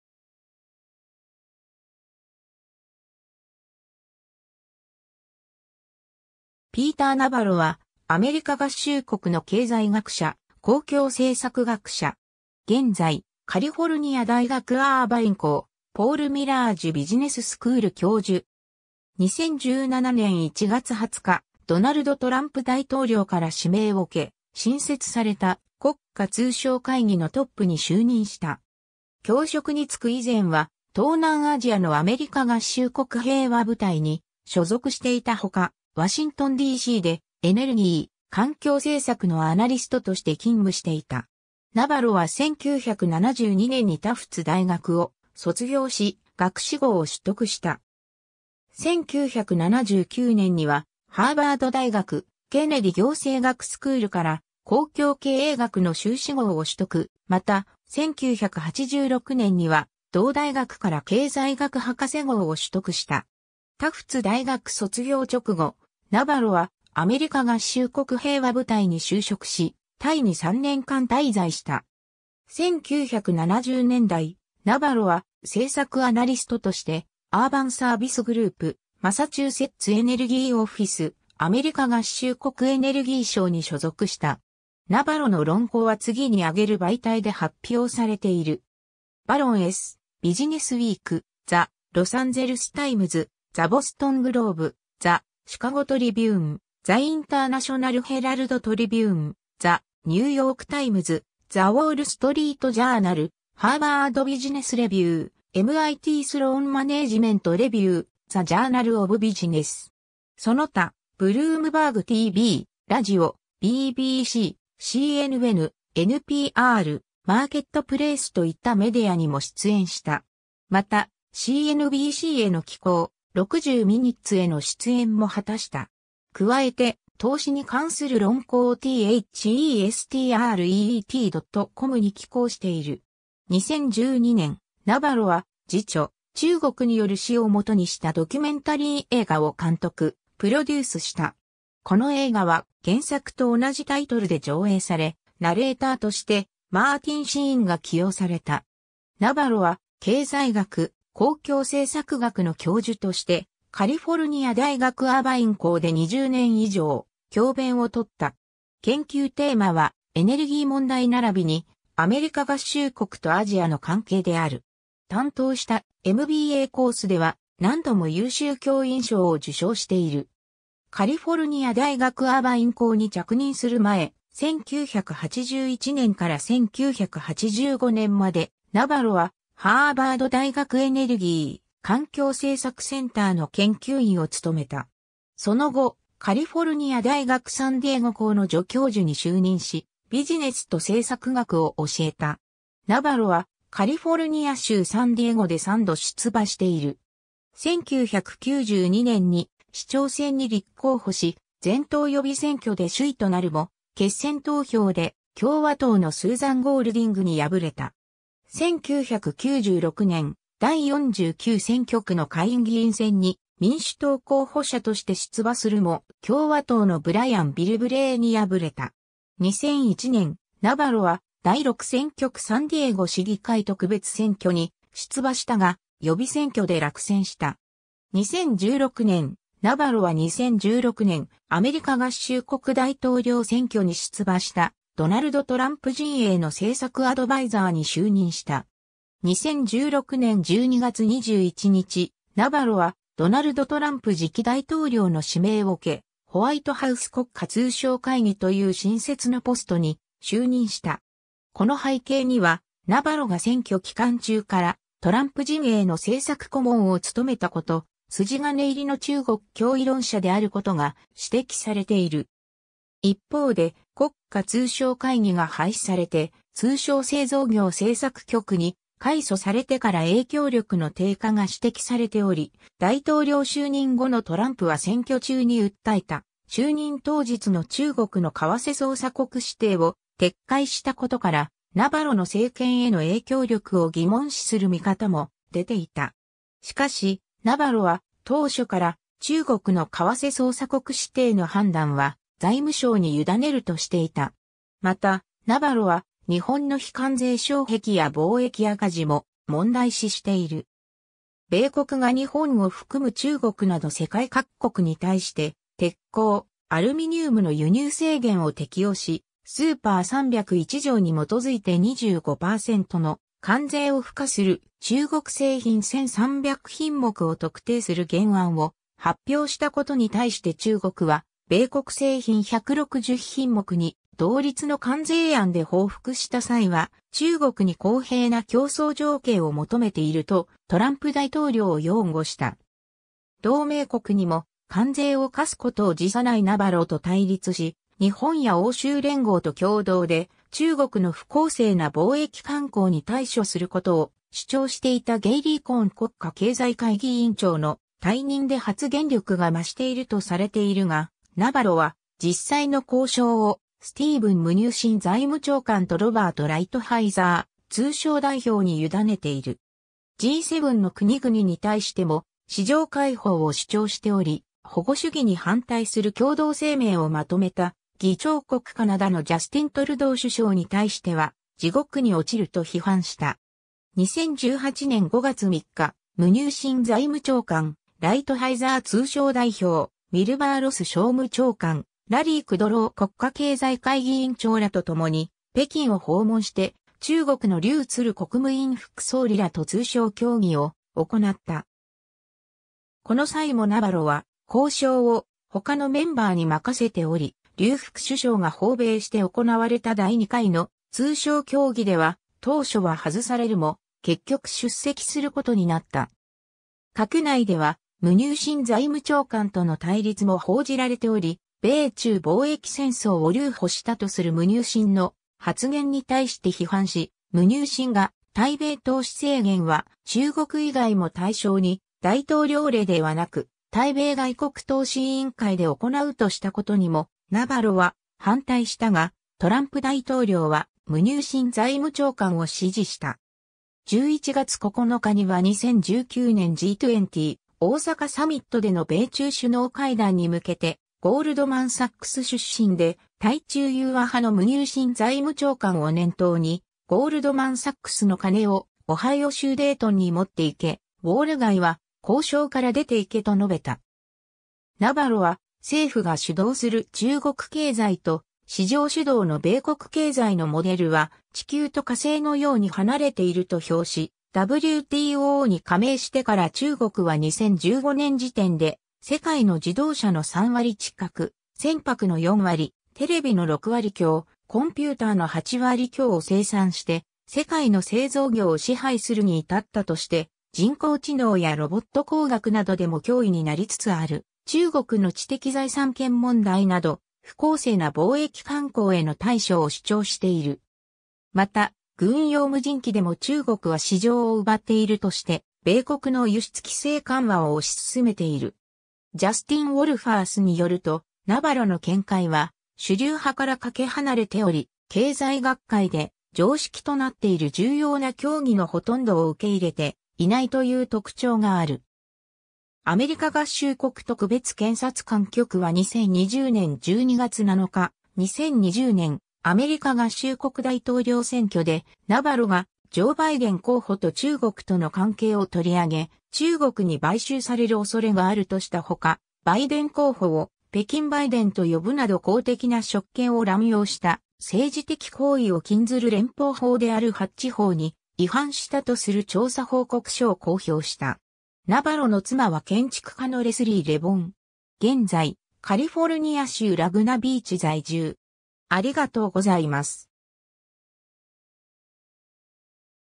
ピーター・ナヴァロ
ピーター・ナヴァロ（Peter Navarro, 1949年7月15日 - ）は、アメリカ合衆国の経済学者・公共政策学者。
現在、カリフォルニア大学アーバイン校ポールミラージュ・ビジネススクール教授。
2017年1月20日、ドナルド・トランプ大統領から指名を受け、新設された国家通商会議（現・通商製造業政策局）のトップに就任した。
 教職に付く以前は東南アジアのアメリカ合衆国平和部隊に所属していたほか、ワシントンD.C.でエネルギー・環境政策のアナリストとして勤務していた。
 ナヴァロは1972年にタフツ大学を卒業し、学士号を取得した。
1979年にはハーバード大学ケネディ行政学スクールから公共経営学の修士号を取得、また、1986年には同大学から経済学博士号を取得した。
タフツ大学卒業直後、ナヴァロはアメリカ合衆国平和部隊に就職し、タイに3年間滞在した。
 1970年代、ナヴァロは政策アナリストとしてアーバンサービスグループ、マサチューセッツエネルギーオフィス、アメリカ合衆国エネルギー省に所属した。
 ナヴァロの論考は次に挙げる媒体で発表されている。
『Barron’s』、『Business Week』、『The Los Angeles Times』、『The Boston Globe』、『The Chicago Tribune』、『The International Herald Tribune』、『The New York Times』、『The Wall Street Journal』、『Harvard Business Review』、『MIT Sloan Management Review』、『The Journal of Business』。
 その他、Bloomberg TV、ラジオ、BBC、CNN、NPR、Marketplaceといったメディアにも出演した。
また、CNBCへの寄稿、60 Minutesへの出演も果たした。
加えて、投資に関する論考をthestreet.comに寄稿している。
 2012年、ナヴァロは自著『中国による死（Death by China）』をもとにしたドキュメンタリー映画を監督・プロデュースした。
この映画は原作と同じタイトルで上映され、ナレーターとしてマーティン・シーンが起用された。
 ナヴァロは経済学・公共政策学の教授としてカリフォルニア大学アーバイン校で20年以上教鞭をとった。
研究テーマは、エネルギー問題ならびにアメリカ合衆国とアジアの関係である。
担当したMBAコースでは、何度も優秀教員賞を受賞している。
 カリフォルニア大学アーバイン校に着任する前、1981年から1985年まで、ナヴァロはハーバード大学エネルギー・環境政策センターの研究員を務めた。
その後、カリフォルニア大学サンディエゴ校の助教授に就任し、ビジネスと政策学を教えた。
 ナヴァロはカリフォルニア州サンディエゴで3度出馬している。
1992年に市長選に立候補し、全党予備選挙で首位となるも、決選投票で共和党のスーザン・ゴールディング(Susan Golding)に敗れた。
1996年、第49選挙区の下院議員選に民主党候補者として出馬するも、共和党のブライアン・ビルブレイ(Brian Bilbray)に敗れた。
2001年、ナヴァロは第6選挙区サンディエゴ市議会特別選挙に出馬したが、予備選挙で落選した。
 2016年、ナヴァロは2016年アメリカ合衆国大統領選挙に出馬したドナルド・トランプ陣営の政策アドバイザーに就任した (en)。
2016年12月21日、ナヴァロはドナルド・トランプ次期大統領の指名を受け、ホワイトハウス国家通商会議（NTC）という新設のポストに就任した。
この背景には、ナヴァロが選挙期間中からトランプ陣営の政策顧問を務めたこと、筋金入りの中国脅威論者であることが指摘されている。
一方で国家通商会議が廃止されて「通商製造業政策局」（OTMP）に改組されてから影響力の低下が指摘されており、大統領就任後のトランプは選挙中に訴えた就任当日の中国の為替操作国指定を撤回したことからナヴァロの政権への影響力を疑問視する見方も出ていた。
しかし、ナヴァロは当初から中国の為替操作国指...